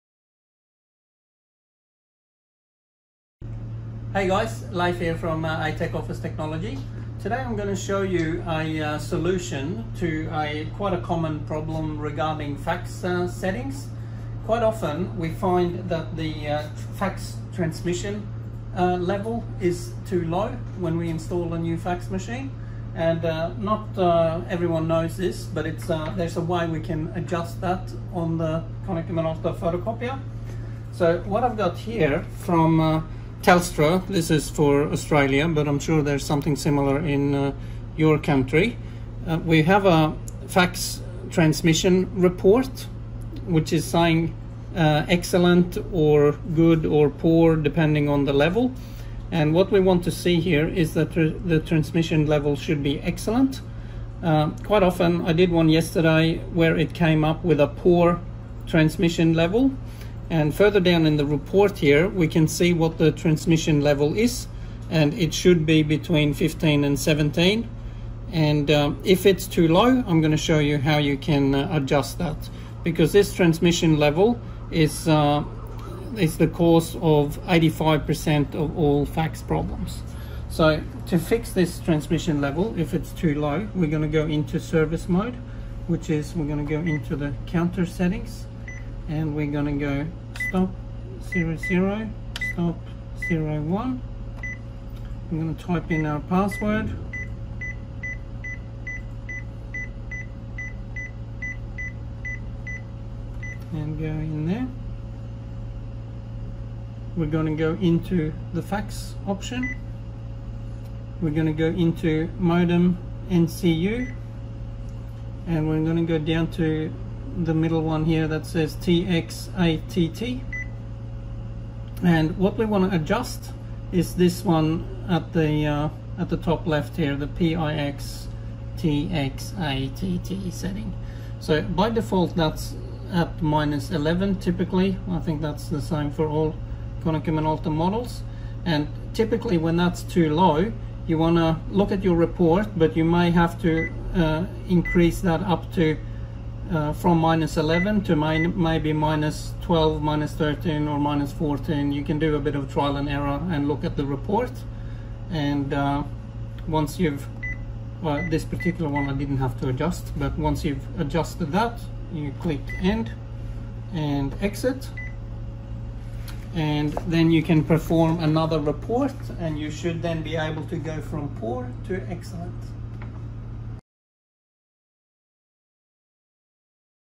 How to Adjust Fax Transmission Level on Konica Minolta MFD
How to adjust fax transmission level on Konica Minolta MFD. According to Telstra, this is the cause of 85% of fax transmission problems in Australia. NOTE: This tutorial is a bit technical, you need access to Service Mode to make this adjustment.